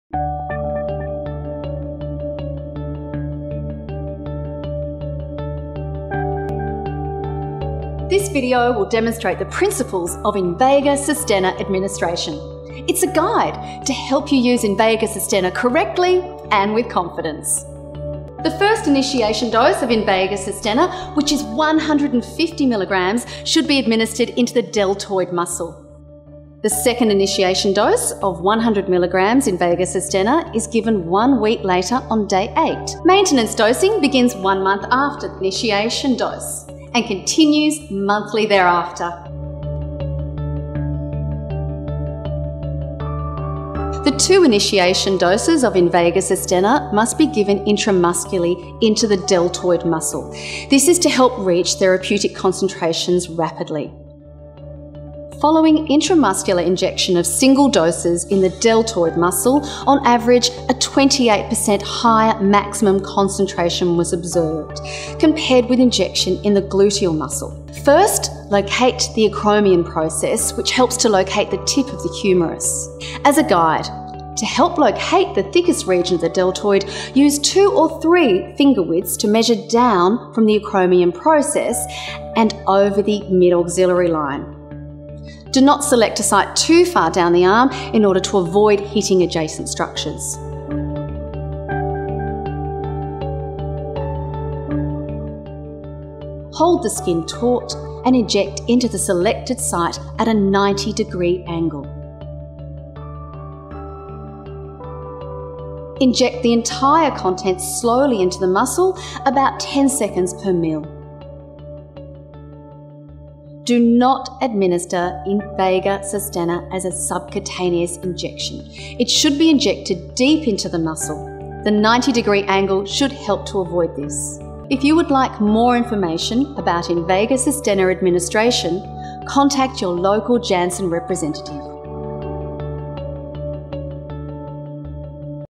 Invega Sustenna - How to Video
Invega Sustenna how to video shot for Janssen, Johnson & Johnson. Multi-camera studio shoot.